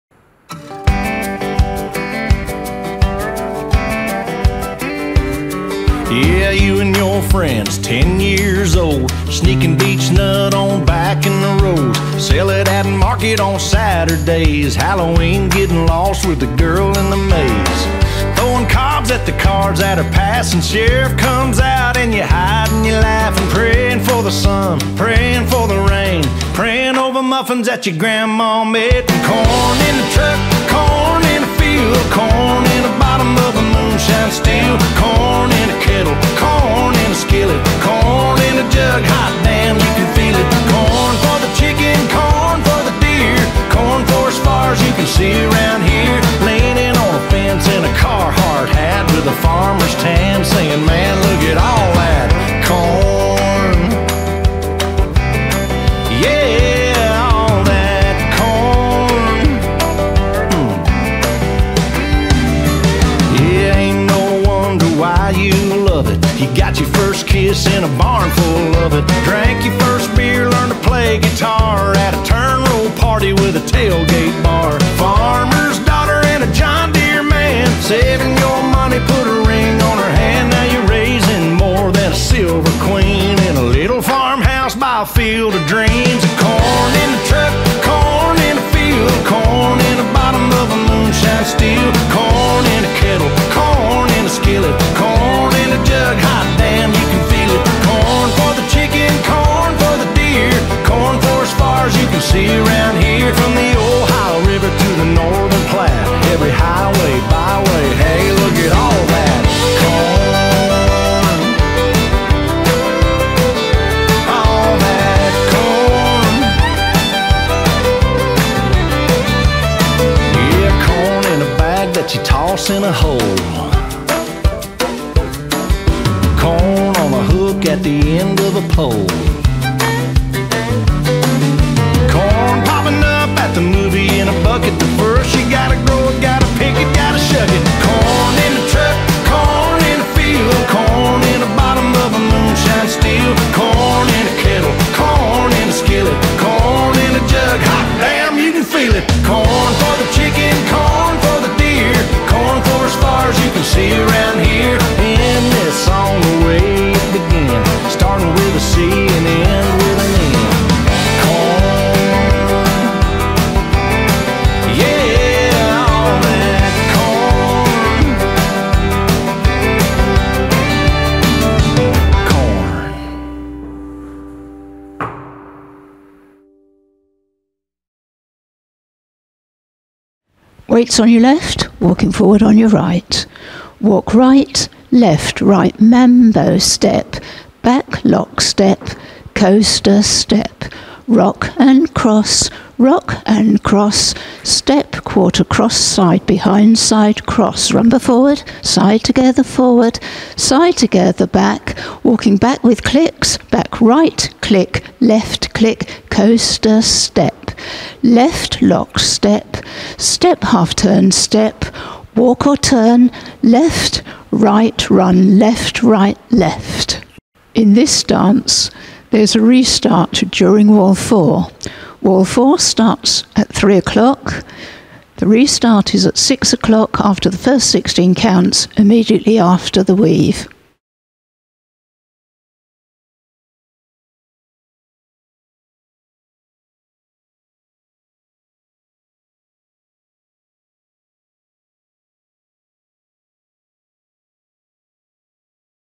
CORN - Line Dance Demo & Walk Through
CORN- LINE DANCE
Jump to 02:57 for a quick walk through.

CORN is a classic traditional line dance.

Just dance and enjoy.
A quick walk through is given at the end of the video as a reminder of the steps.

CORN - 32 counts/4 walls  -  choreographed by Rob Fowler

for details of A B Coasters Linedance classes.